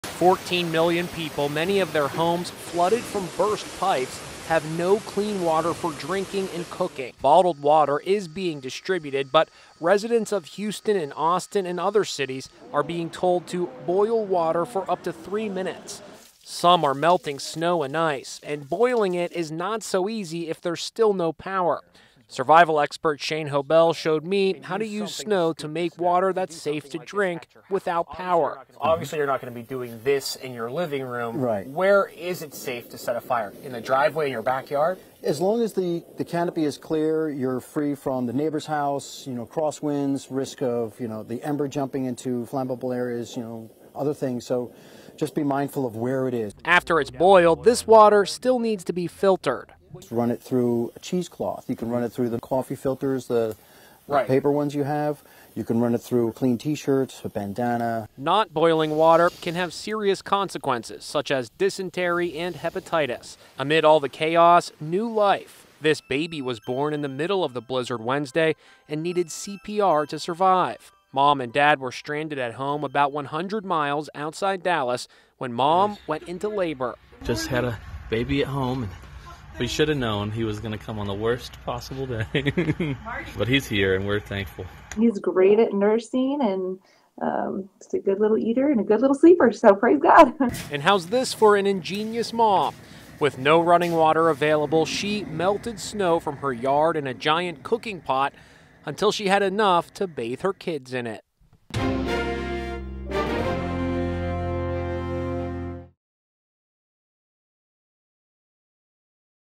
Is Melted Snow Safe to Drink?
Millions of Texans are still under orders to boil water as the winter blast continues to take its toll. But if your pipes are frozen, is it safe to drink melted snow? Fourteen million people, many with homes flooded from burst pipes, have no clean water for drinking and cooking. Broken water mains and power blackouts have made the water supply undrinkable. Bottled water is being distributed, but residents of Houston, Austin and other cities are being told to boil water for up to three minutes.